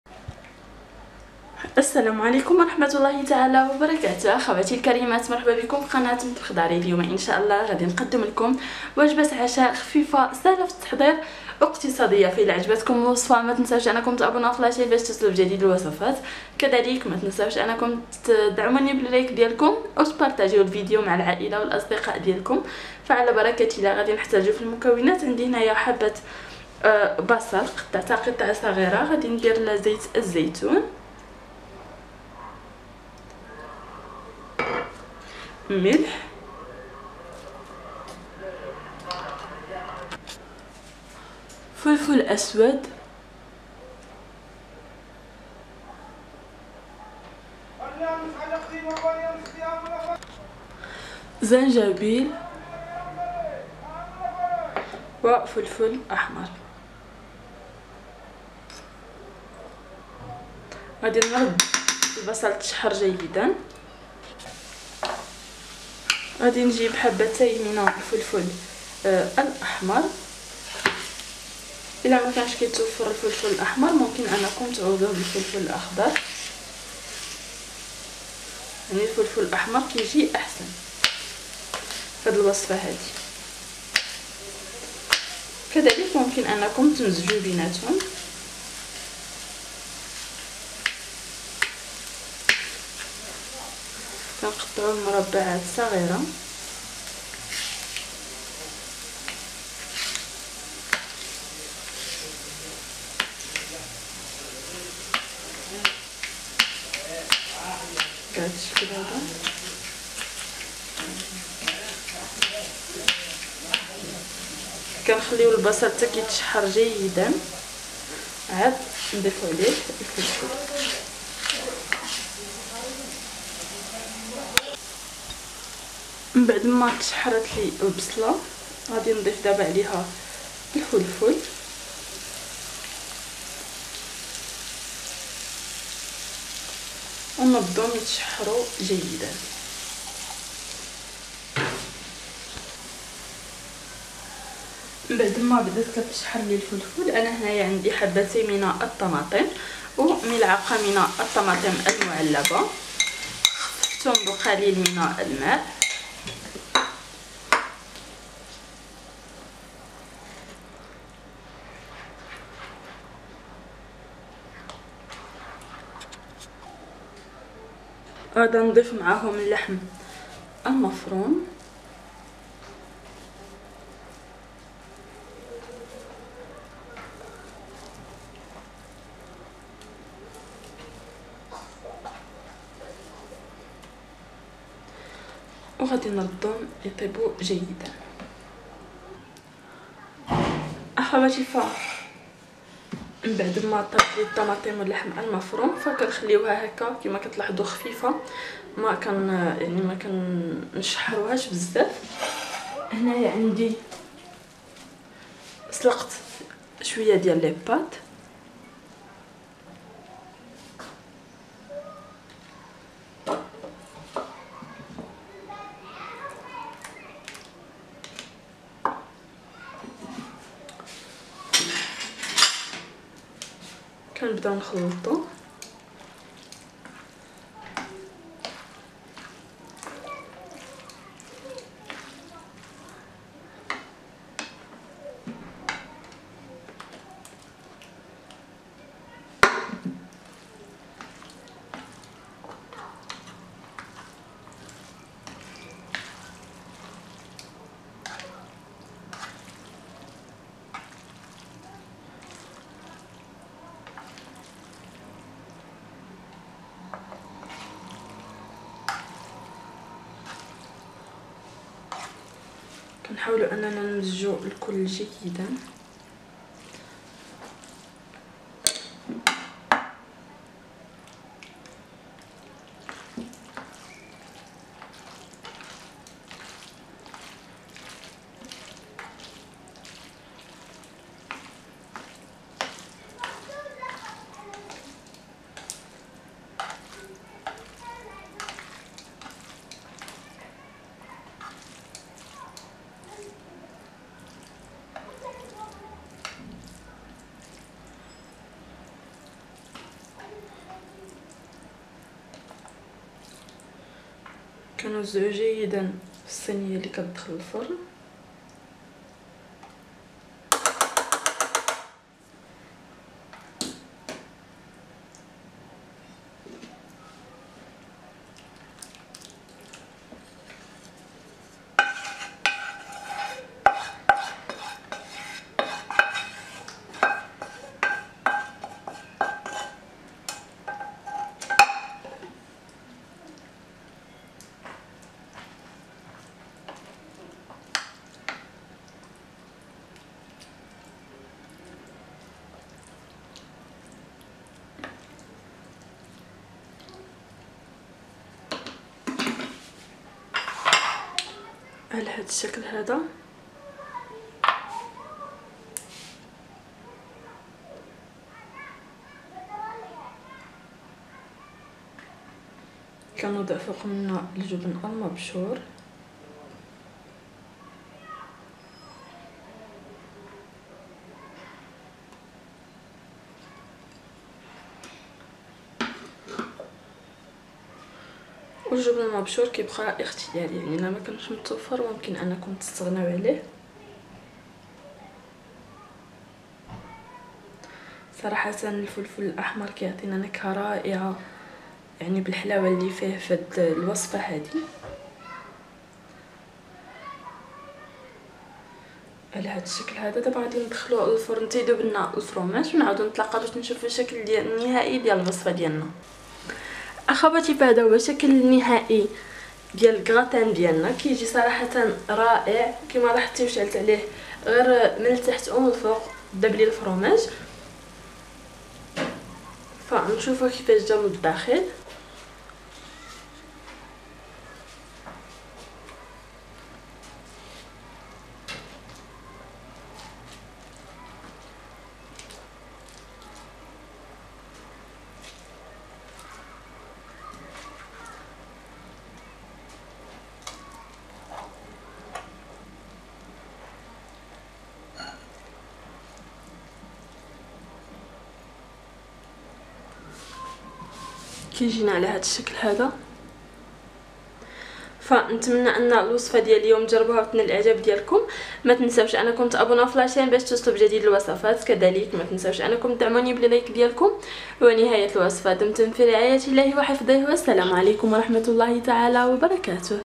وجبة عشاء تحفه البيت كله هيشكرك عليها وسهله جدا | كراتان سهل التحضير و إقتصادي👌
كيك يومي ببيضة واحدة فقط اقتصادي و سهل التحضير و بحجم عائلي ابهري بهطفالك بعد العودة من المدرسة